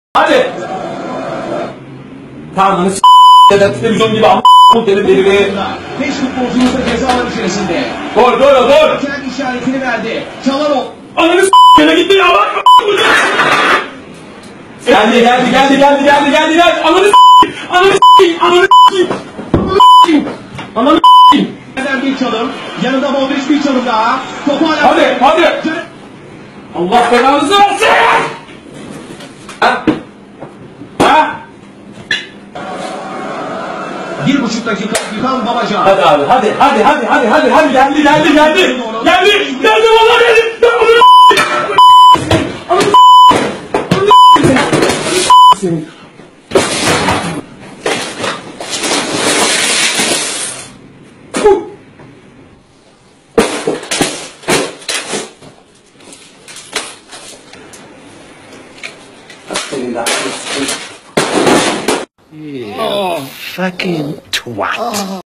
Wife Pranks Husband - Hilarious
Girlfriend pranks Turkish football fan by using an invisible remote at every moment of importance.

Just watch what he does?